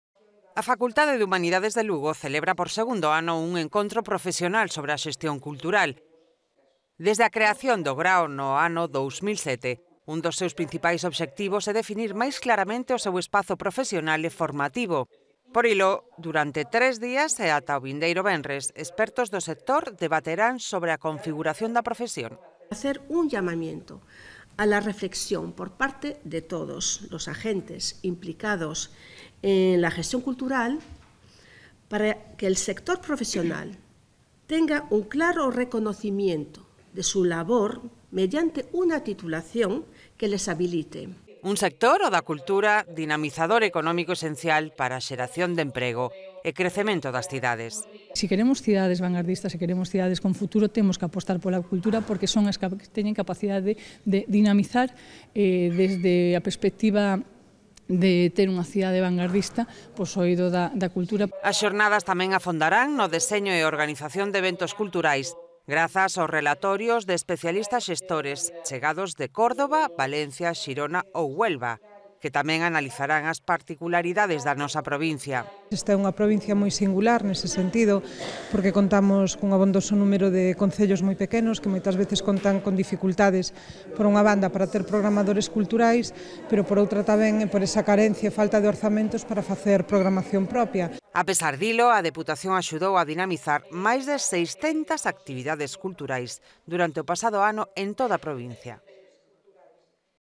A Xestión Cultural, a debate na Facultade de Humanidades de Lugo
A Facultade de Humanidades de Lugo celebra, por segundo ano, un econtro profesional sobre a Xestión Cultural. Dende a creación do grao no ano 2007, un dos seus principais obxectivos é definir, craramente, o seu espazo profesional e formativo.